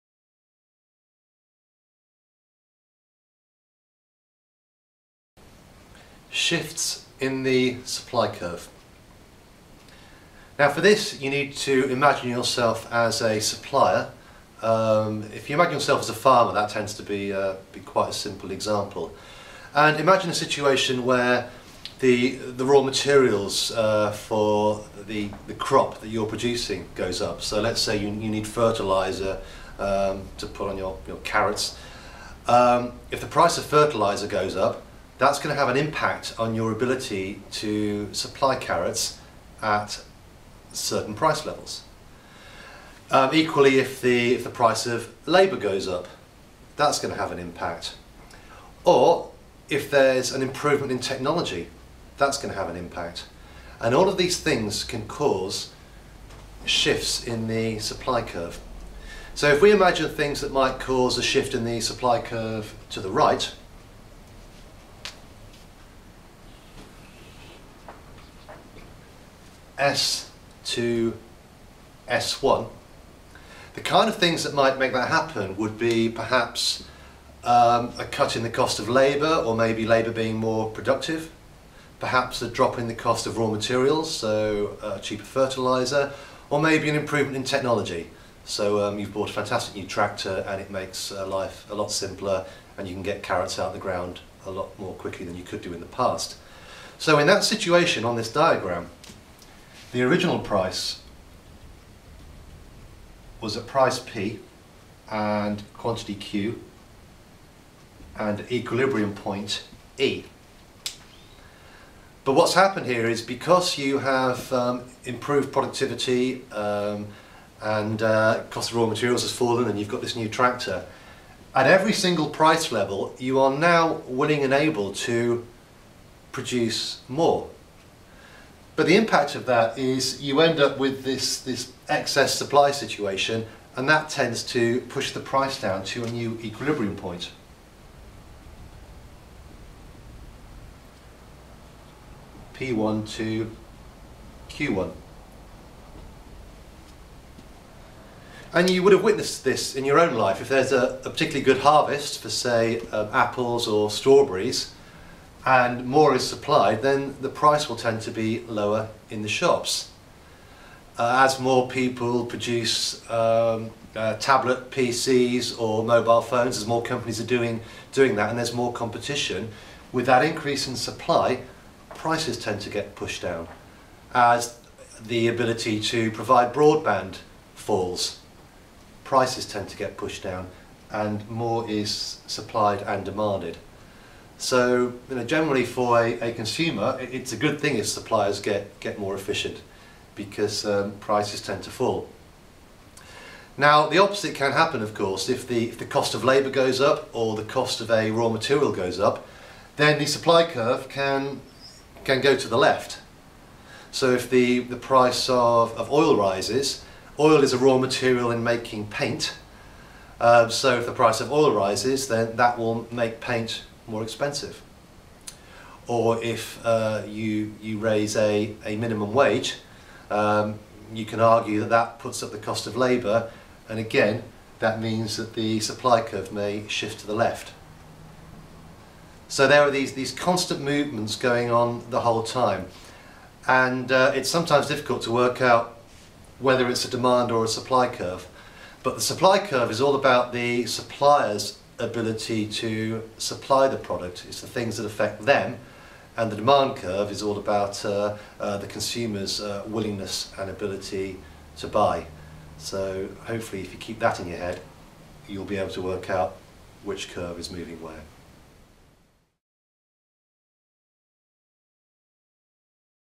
Shifts in the Supply Curve.wmv
An informal introduction to shifts in the supply curve on a supply and demand diagram.  - teacher, lecturer and speaker Nick Langston-Able gives a brief explanation on supply and demand, the supply curve, and the factors which could shift the supply curve to the right including changes in the price of raw materials, cost of labour and changes in technology.  He also refers to the the equilibrium point, excess demand, excess supply and the market mechanism.